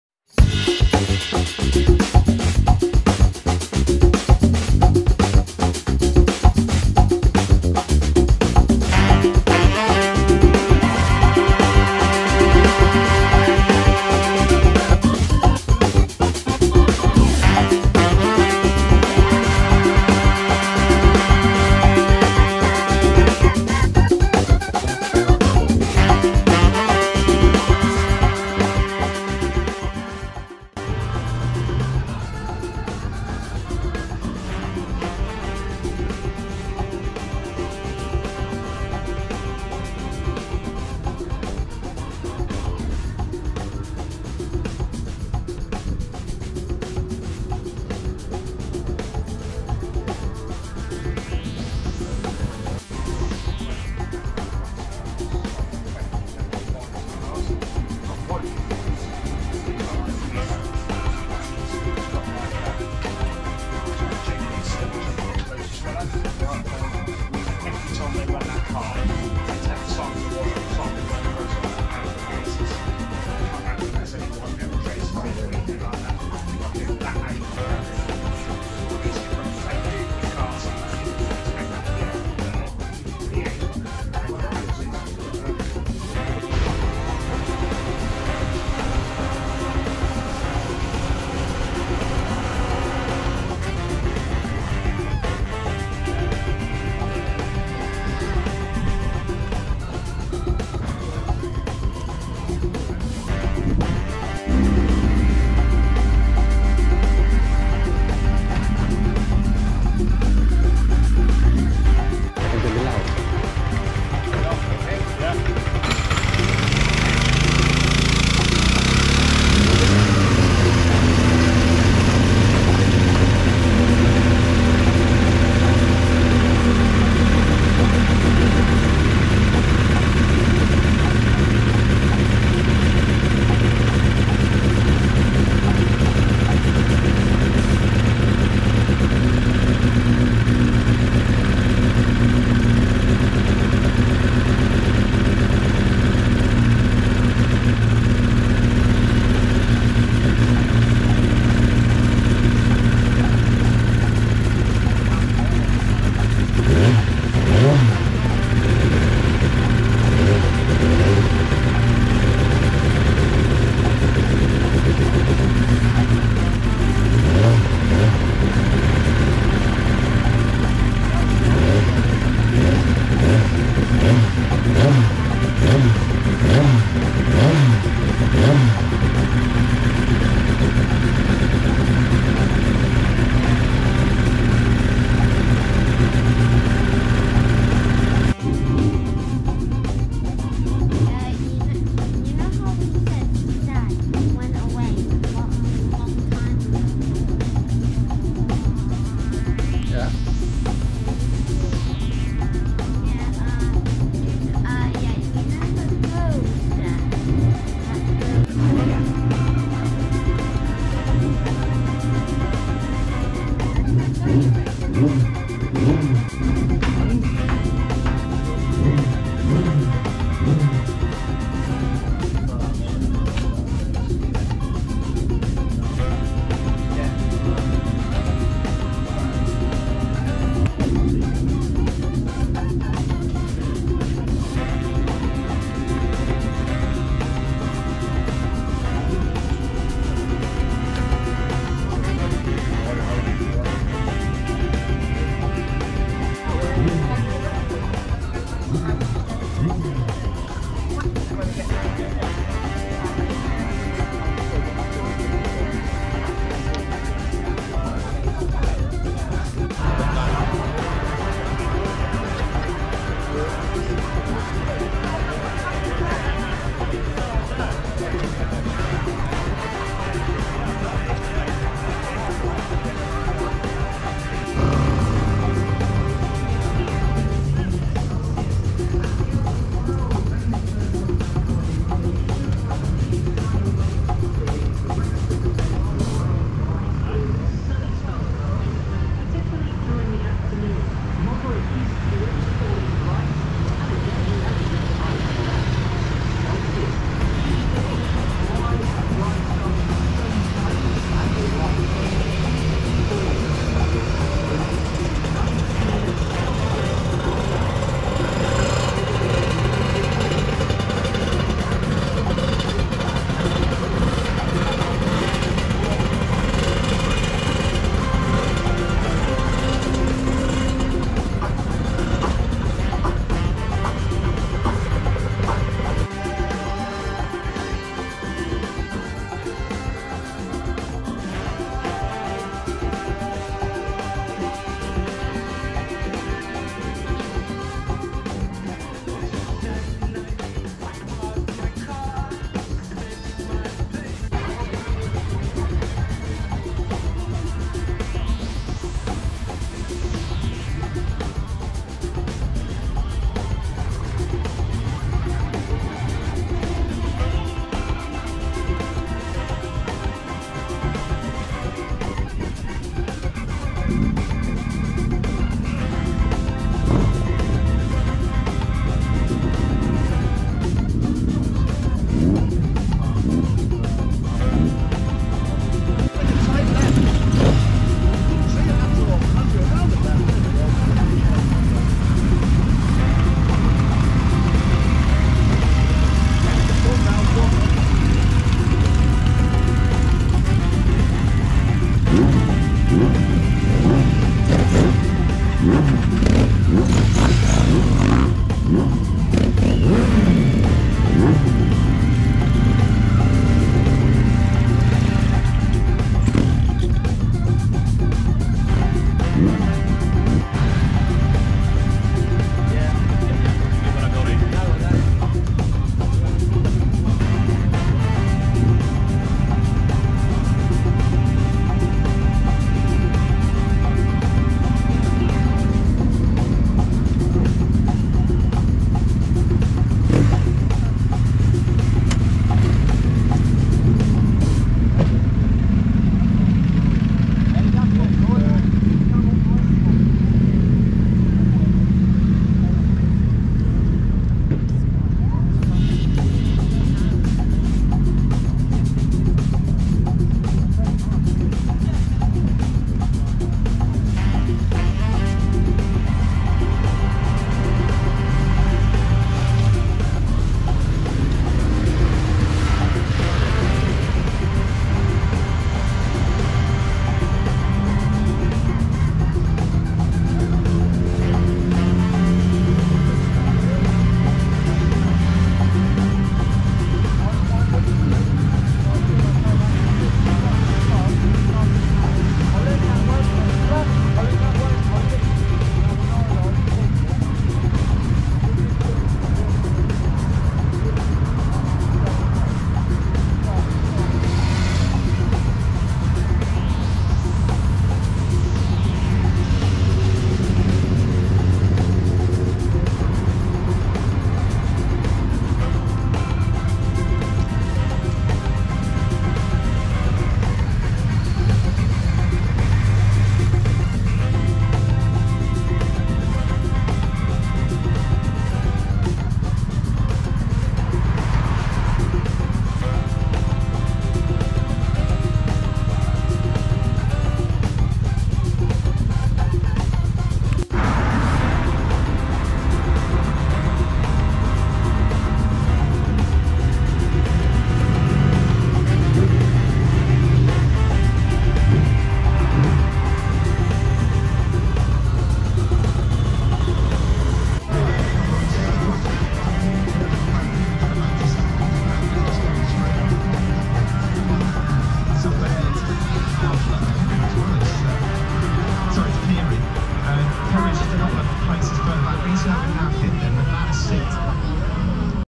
The Manx Grand Prix 2022 Paddock Walk 28082022
The 2022 Manx Grand Prix is in full swing, and I missed the first week of it :(
But I'm here now so enjoy a paddock walk from a very quiet Sunday 28th August 2022.
Explore the paddocks, watch the bikes from the grandstand and walk up the pit lane.

Thanks.